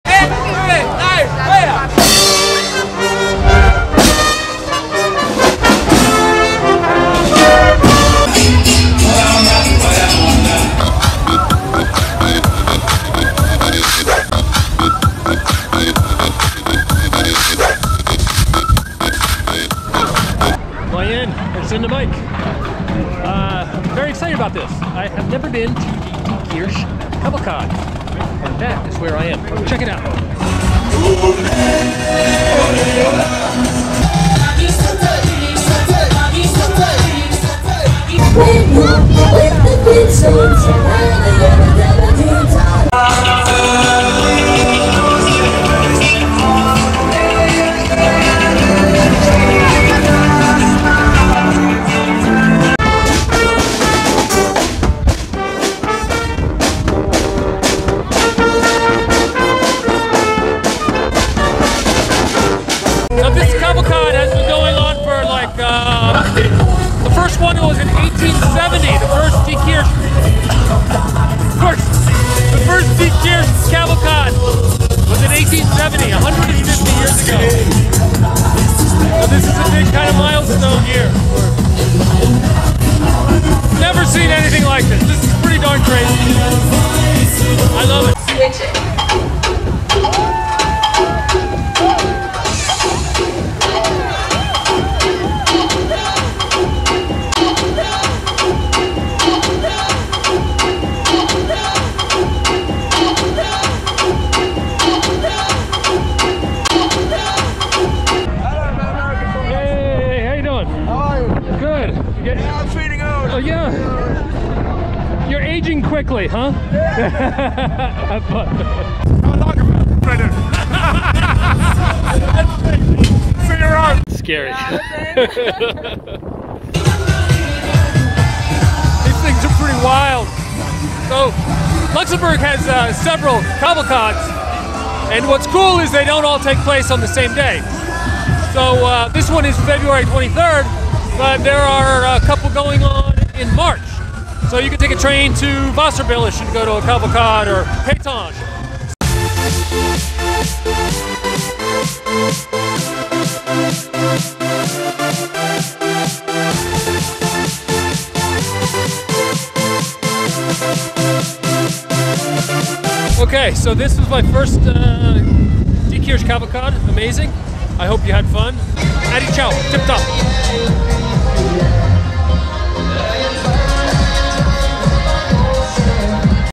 Diekirch Cavalcade 2020 - An American in Luxembourg
The Cavalcade in Diekirch, Luxembourg, has been going in since 1870. Check out this 150th Anniversary edition.

For more, go to An American in Luxembourg on Instagram and Facebook. Or buy my book, 'An American in Luxembourg,' at bookstores throughout Luxembourg or an Amazon.

Thanks, CFL for the ride!